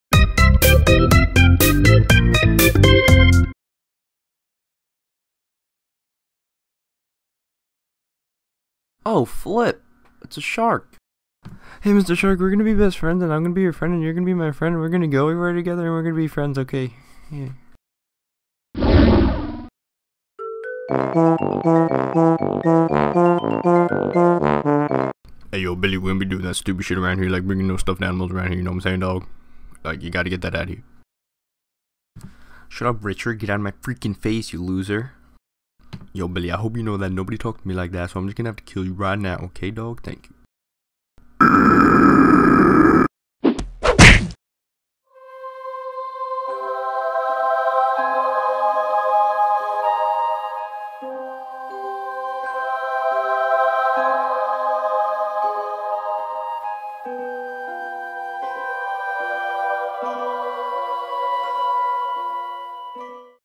Billy and the Shark - Short Animation
Billy and the Shark go on an adventure. The shark is a savage for sure bruh.